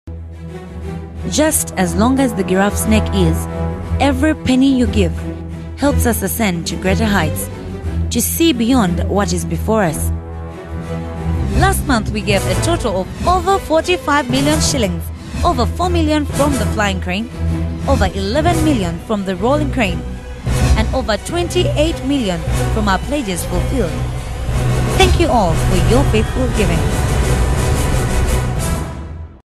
BUILD GODS HOUSE UPDATE.
This is how much we have been able to give towards building God's House, thank you for your faithful giving.